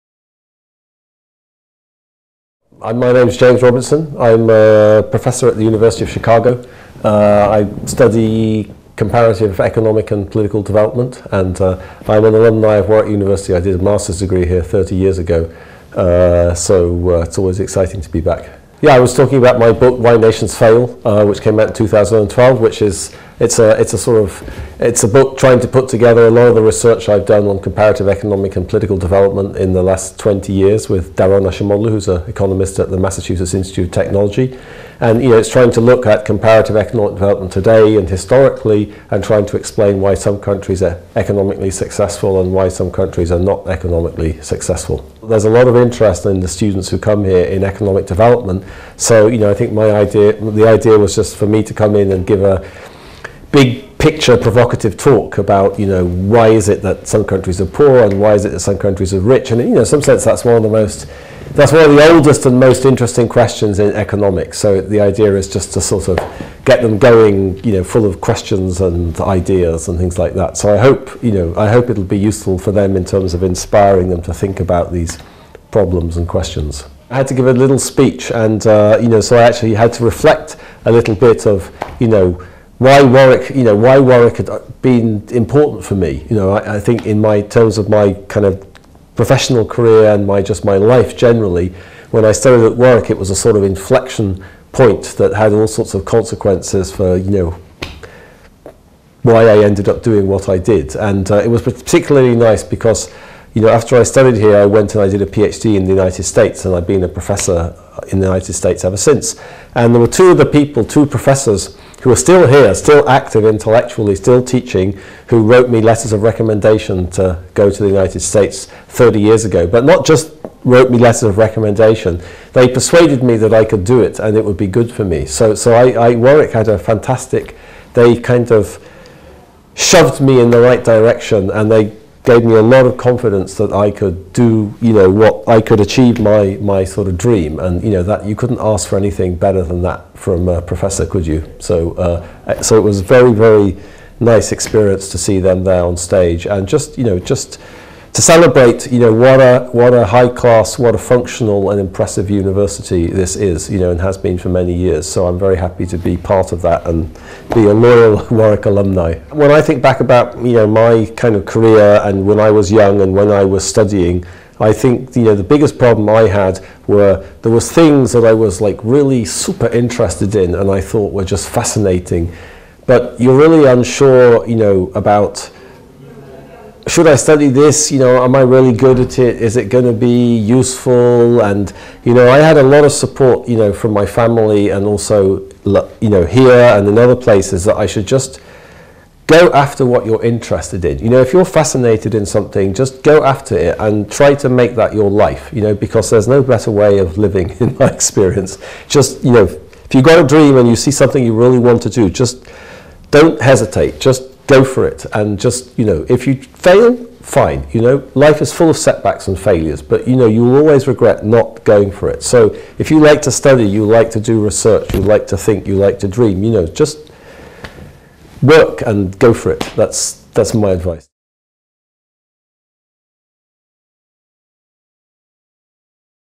Professor James Robinson interview | Warwick Department of Economics
Professor James Robinson visited Warwick this summer to guest lecture at our Summer School and he also received an honorary degree at the summer graduation ceremony.

We caught up with Professor Robinson for a quick chat about his lecture, receiving the honorary degree and to hear a little about his time here at Warwick.

James is a Professor at the University of Chicago and has studied economics at the University of Warwick,  LSE and Yale University and is the author of award winning book ‘Why Nations Fail’.